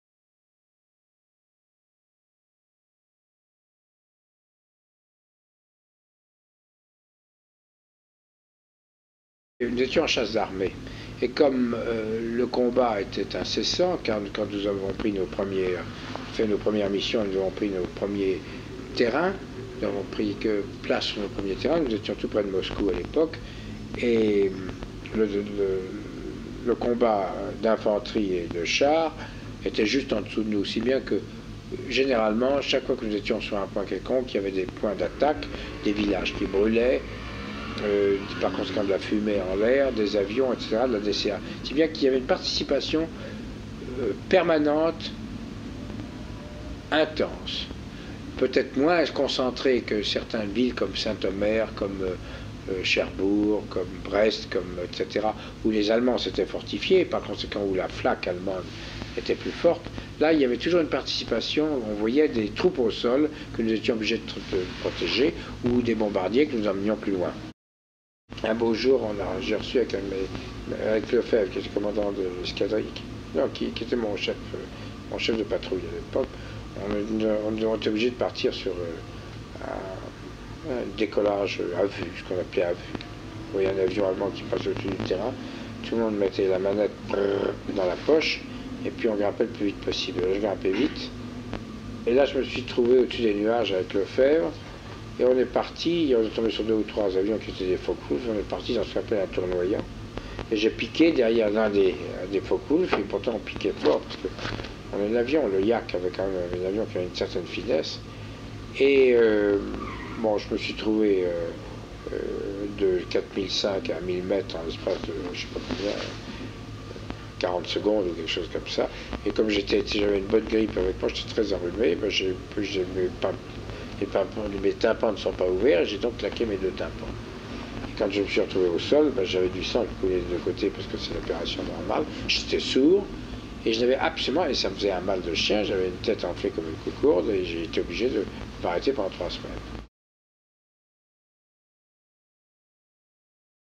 Des aviateurs de la Seconde Guerre mondiale au parcours singulier, Roland de la POYPE.
Témoignage de Roland de la POYPE : (16 victoires aériennes homologuées, 2 probables.
L’extrait de témoignage porte sur les combats de Normandie-Niémen